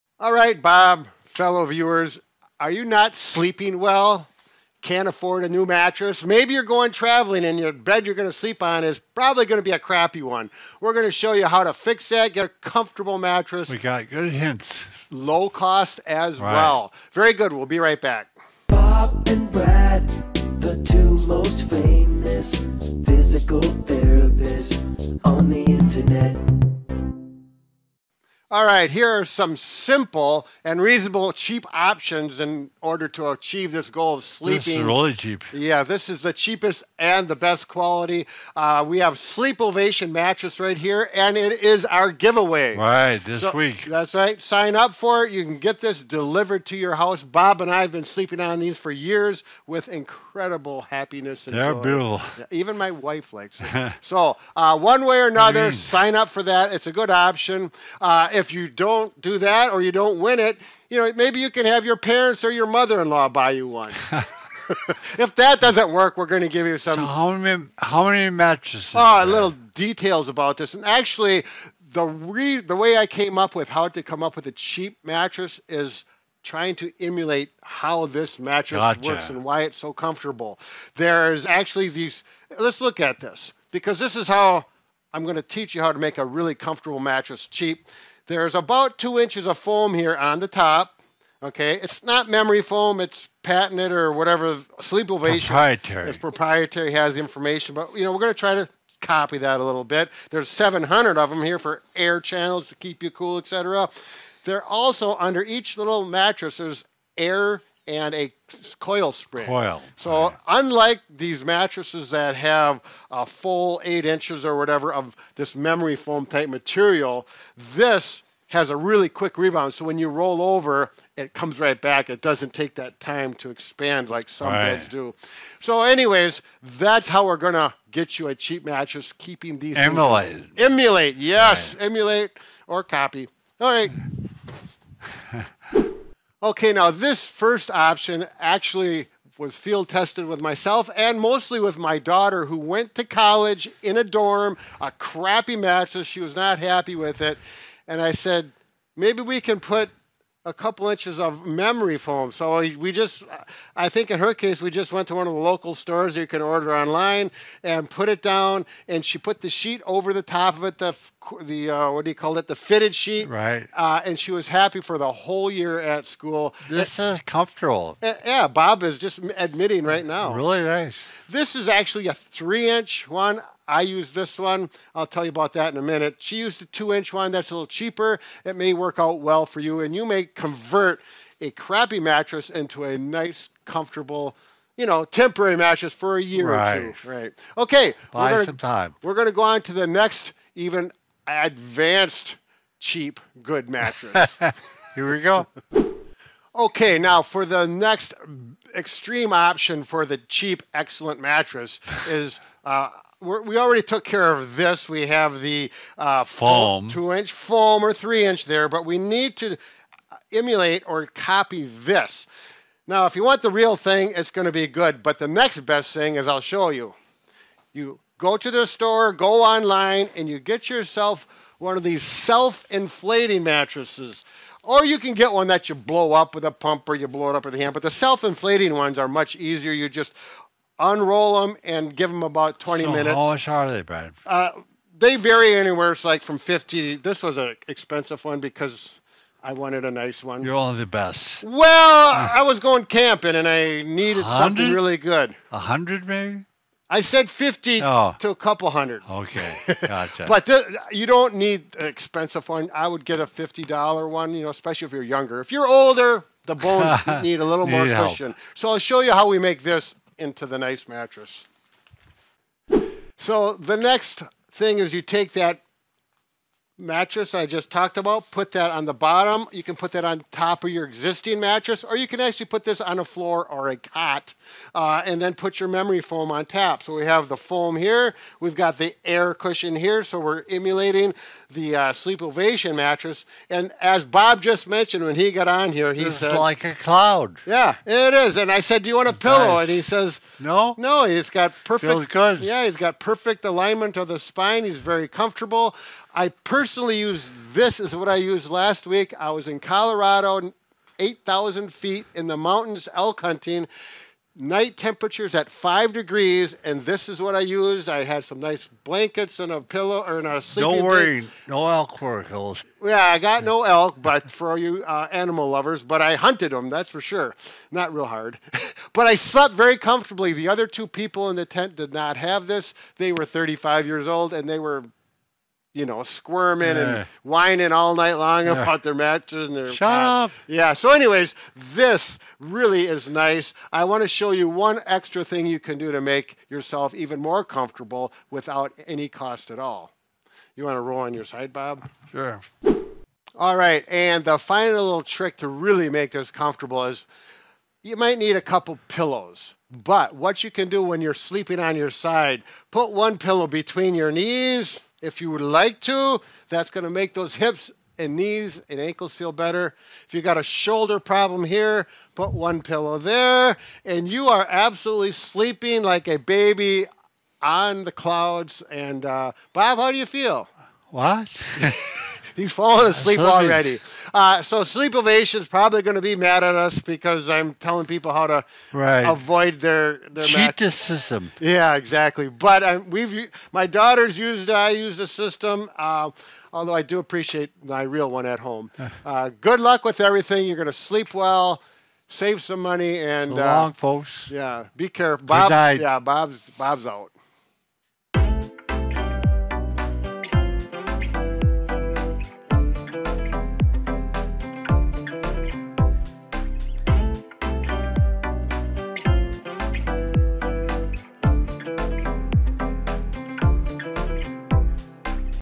Sleep Like A Baby On A Crappy Mattress OR When Traveling (Low Cost)
Bob and Brad demonstrate how to sleep like a baby on a crappy mattress or when traveling at a low cost.

Video Chapters

0:00 intro, 0:28 giveaway, 2:16 how to make the first mattress, 3:16 adding an air mattress, 5:44 adding more pillows to the mattress, 6:15 outro

Featured Products

Our Products:

Pain Management:

Leg Massager Back Massager (Cordless)
Back and Neck Massager with cord
Back and Neck Massager cordless